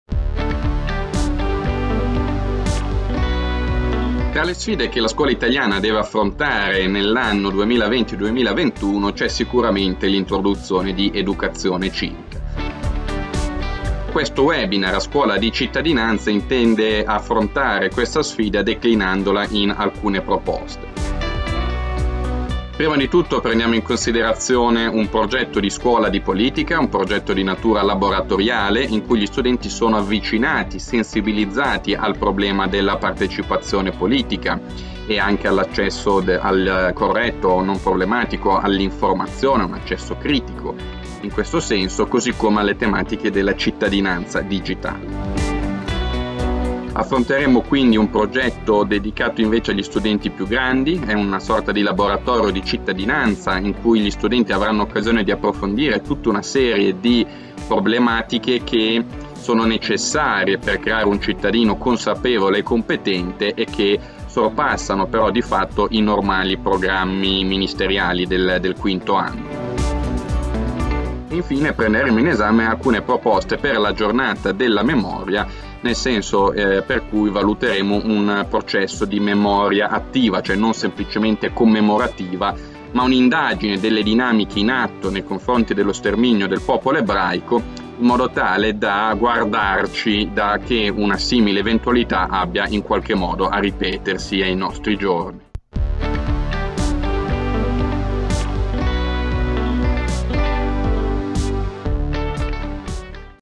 A scuola di cittadinanza | Webinar 7 e 8 settembre 2021
Webinar 7 e 8 settembre 2021
L’insegnamento dell’educazione civica non è da ricondursi allo studio esclusivo della costituzione, ma merita di essere inserito nel quadro più ampio della formazione di giovani cittadini responsabili. In tal senso, all’interno della normativa vigente, saranno presi in considerazione alcune ipotesi di percorsi, vale a dire la scuola di politica, il laboratorio di cittadinanza e la Giornata della Memoria.
A cura di Matteo Bergamaschi e Antonio Famà

VAI ALLA SCHEDA DEL CORSO